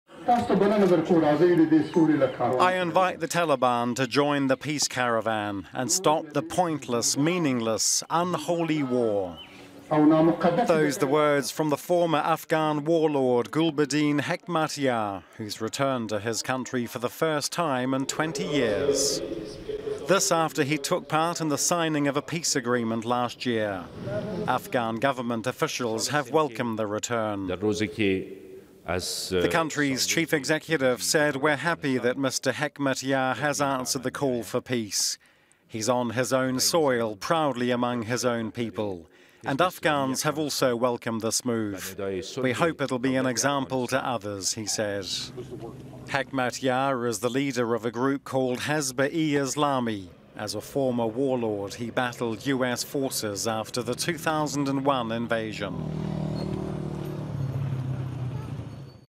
Hezb i Islami Leader G. Hekmatyar returns to Afghanistan calling for peace
By Ahmad Sultan | MEHTAR LAM, AFGHANISTAN
In his first public speech since signing a peace deal with the Afghan government, one of Afghanistan's most notorious warlords on Saturday called for the Taliban to stop fighting and begin negotiations.

"I invite you to join the peace caravan and stop the pointless, meaningless and unholy war," Gulbuddin Hekmatyar said to a gathering of his followers and Afghan politicians in Laghman province, east of the capital, Kabul.

"I want a free, proud, independent and Islamic Afghanistan," he said.

In February the United Nations Security Council agreed to drop sanctions against Hekmatyar, paving the way for him to return openly to Afghanistan.

The Afghan government requested the move as part of a peace deal with Hekmatyar and his militant group, Hezb-i-Islami, in September.